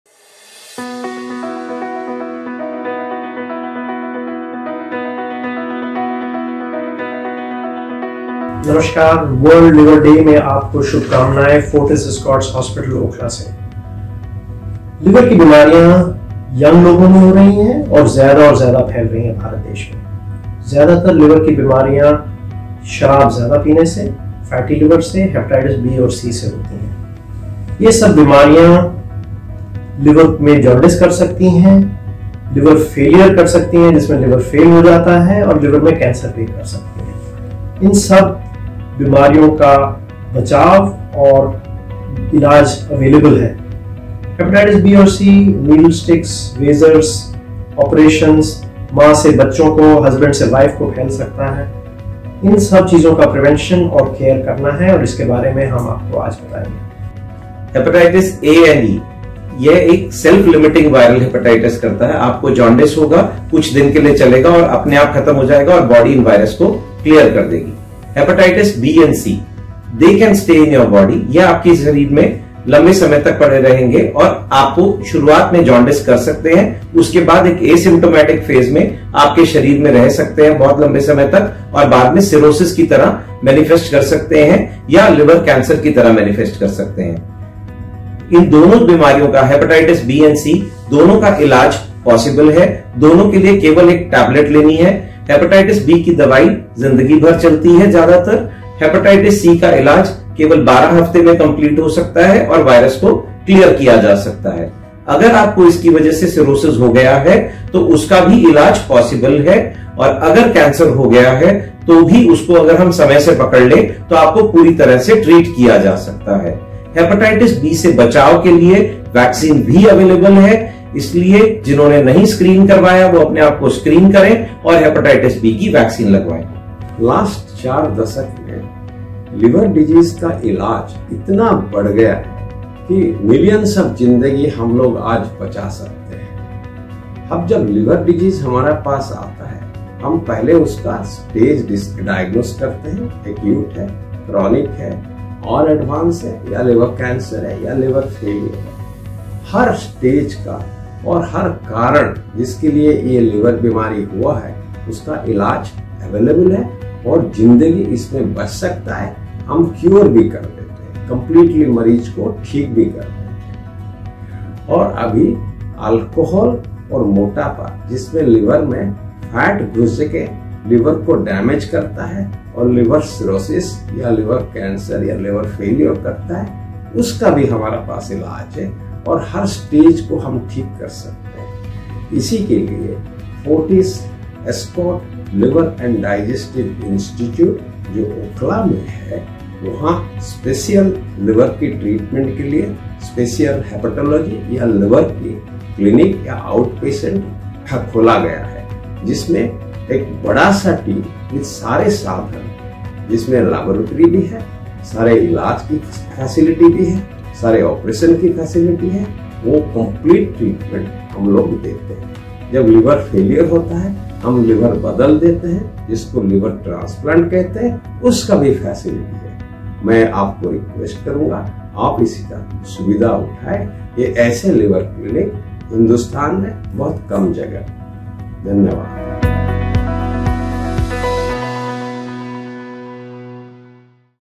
World Liver Day from Fortis Experts.
Dr. Pankaj Puri (Director, Gastroenterology & Hepatobiliary Sciences), Dr. Nishant Nagpal (Senior Consultant, Gastroenterology & Hepatobiliary Sciences), and Dr. Subrat Kumar Acharya (Executive Director, Gastroenterology & Hepatobiliary Sciences) from the Fortis Escorts Liver & Digestive Diseases Institute talk about how you can avoid liver diseases by practicing moderation in your diet and make healthy lifestyle choices on the occasion of World Liver Day.

At Fortis, We Care.